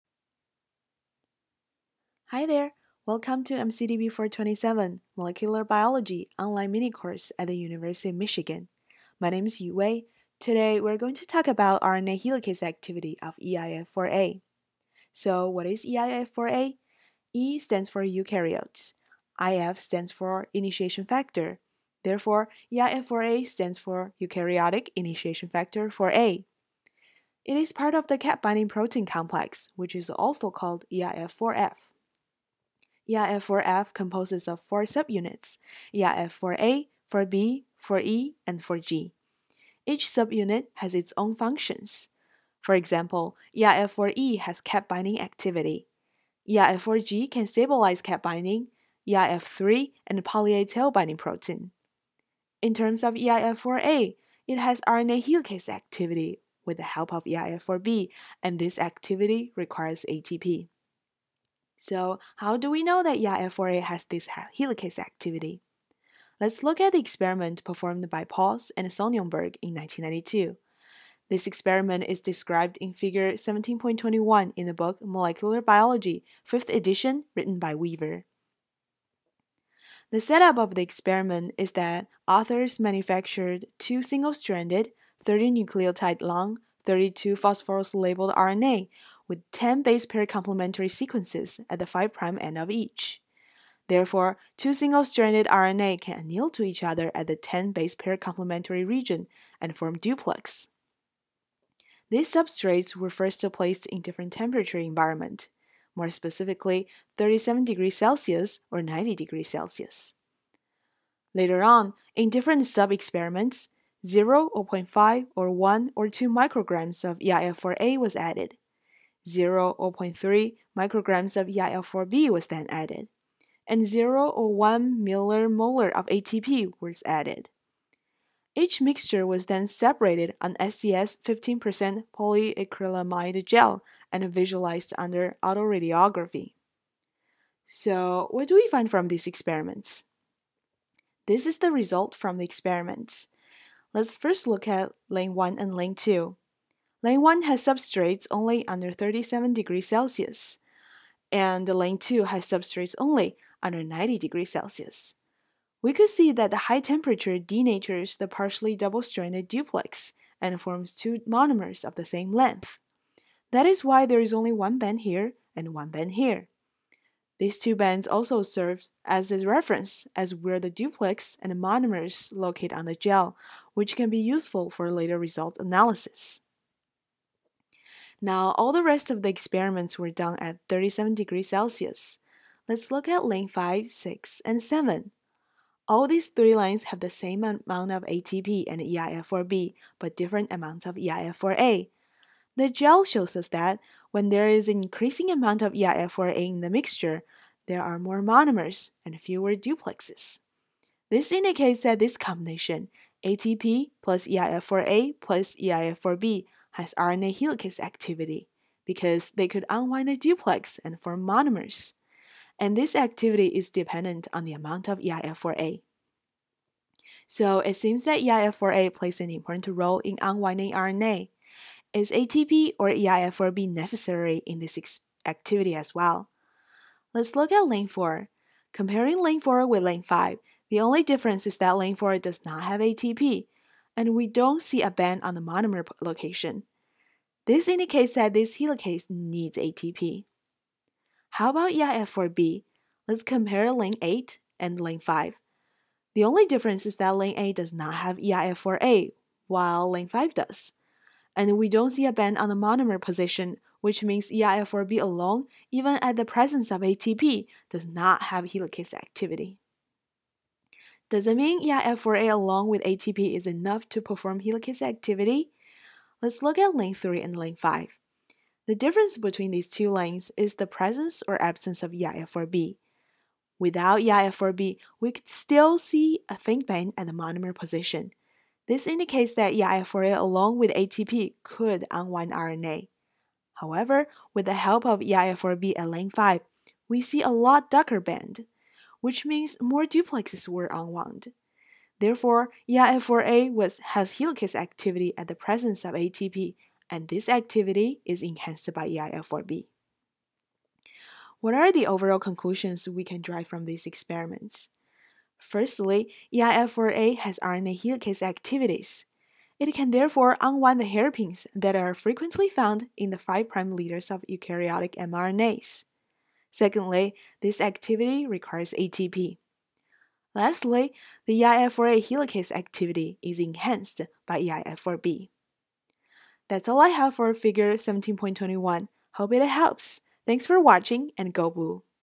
Figure 17.21 eIF4A has helicase activity
This video describes a helicase unwinding experiment that showed that eIF4A is an ATP-dependent helicase and that eIF4B enhances this activity.  This is figure 17.21 from Molecular Biology, 5th edition by Robert Weaver.  This video was made for MCDB 427 at the University of Michigan.